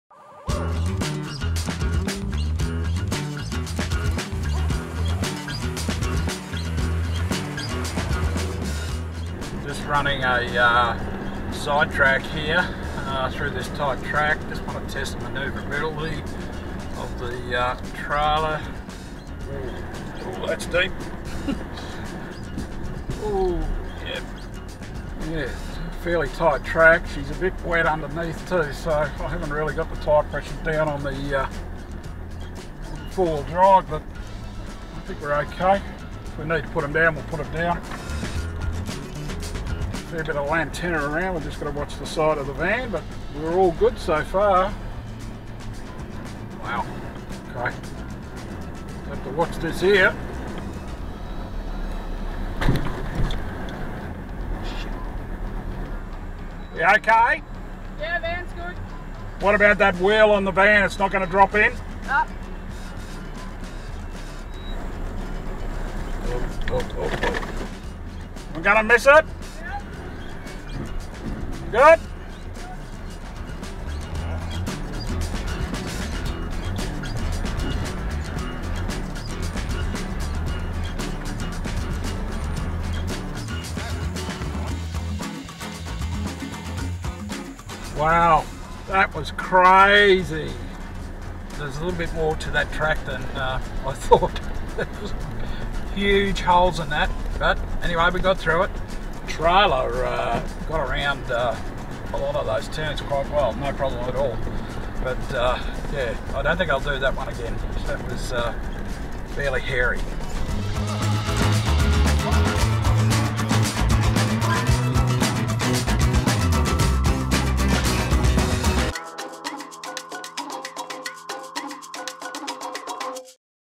WHEN THINGS DON'T QUITE GO TO PLAN 4WDING WITH PLATINUM COMMANDER HYBRID
WHEN THINGS DON'T QUITE GO TO PLAN
Want a serious OFFROAD Hybrid Camper Trailer
Commander Hybrid will do it for you!